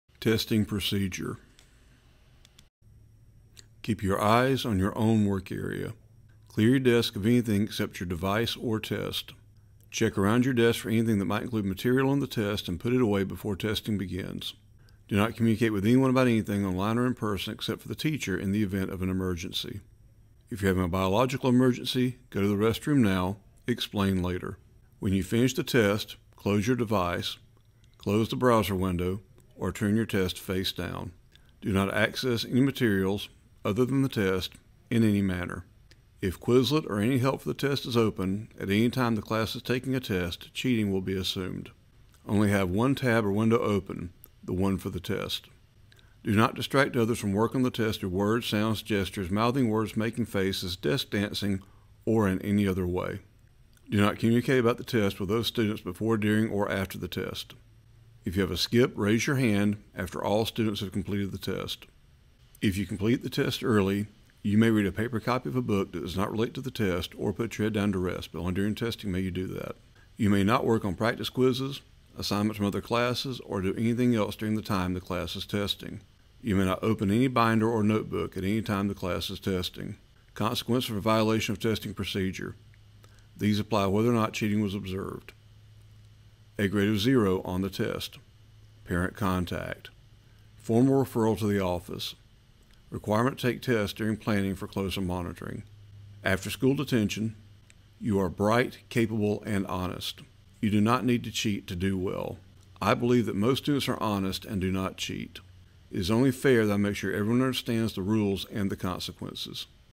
Testing Procedure Video
My rules and procedures for classroom testing. I go over these before each test.